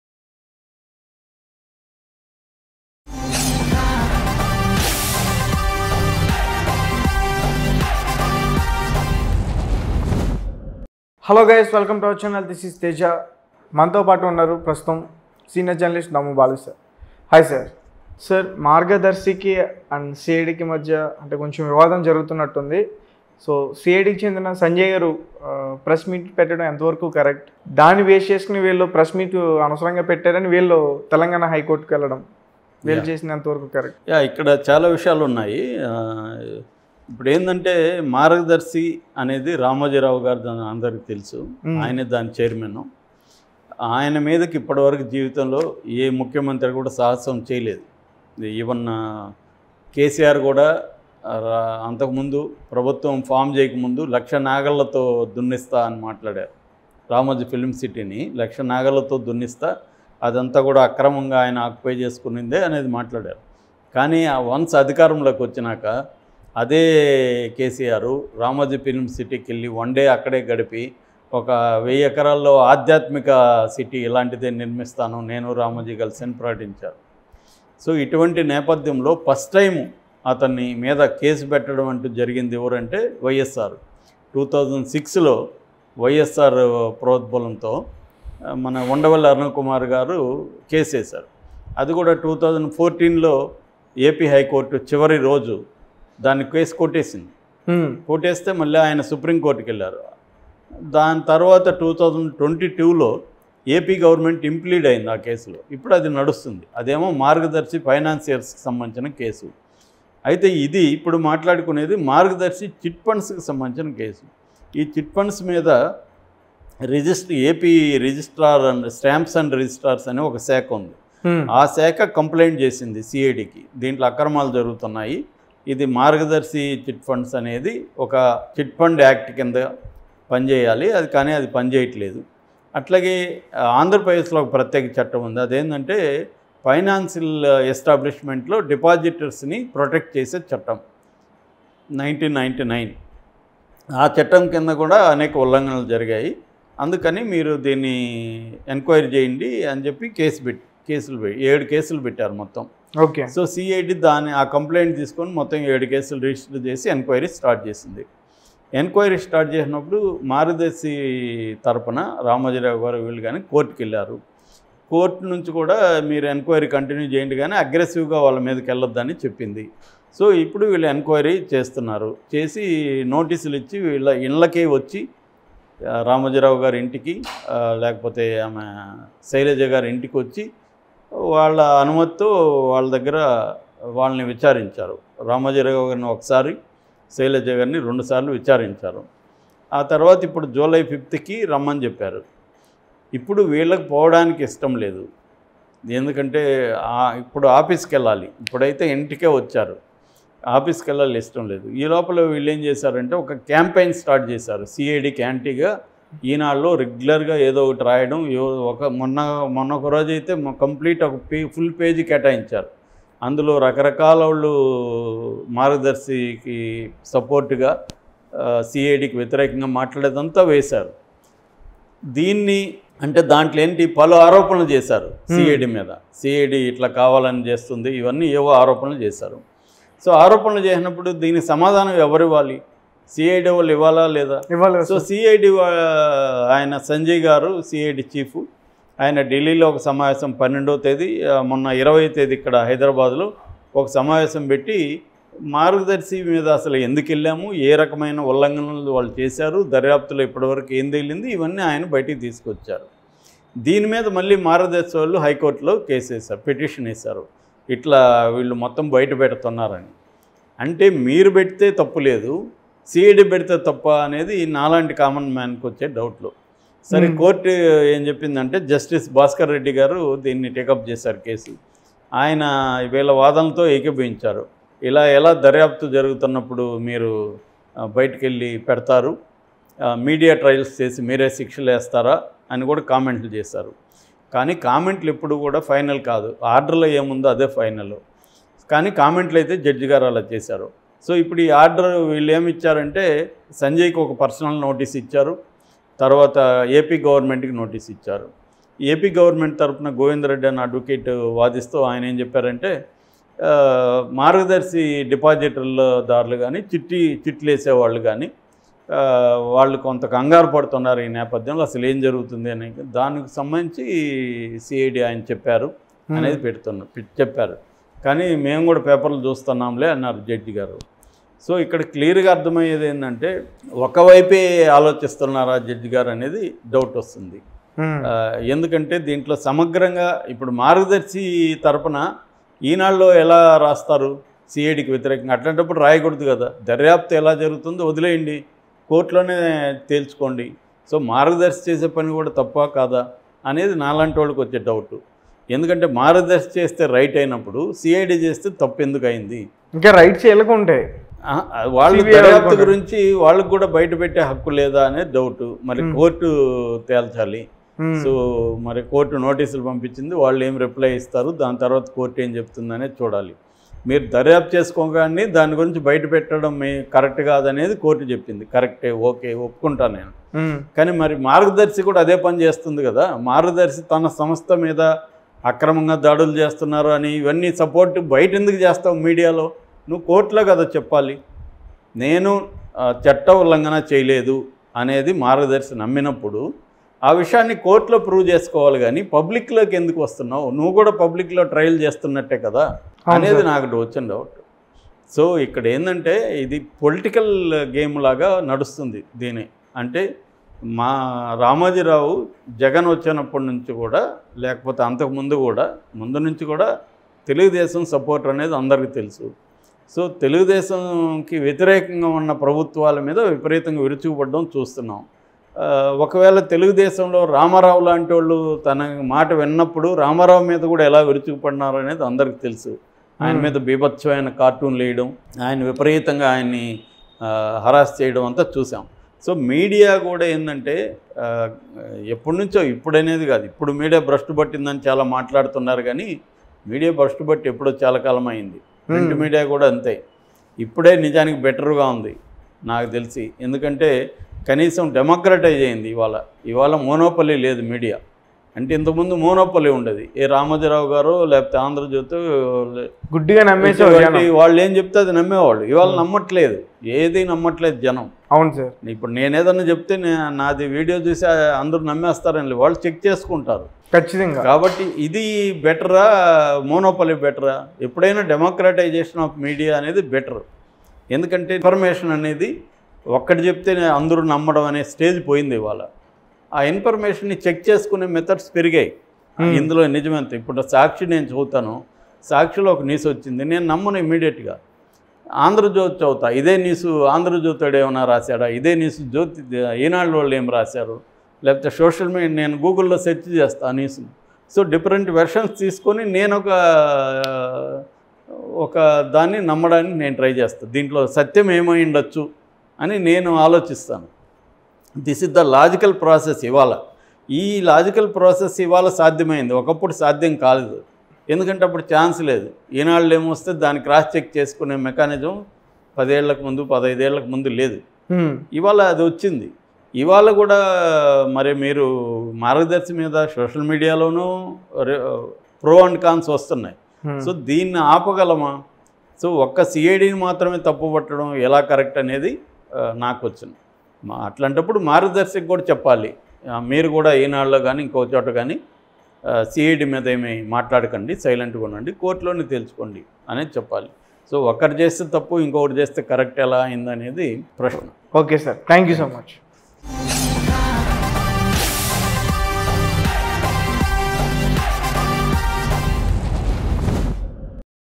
రామోజీ విజయం ఏపీ సీఐడీకి హైకోర్టు షాక్ దర్యాప్తు ఆగినట్టే | Margadarsi and CID Issues CID Sanjay
Daamu Balaji Diaries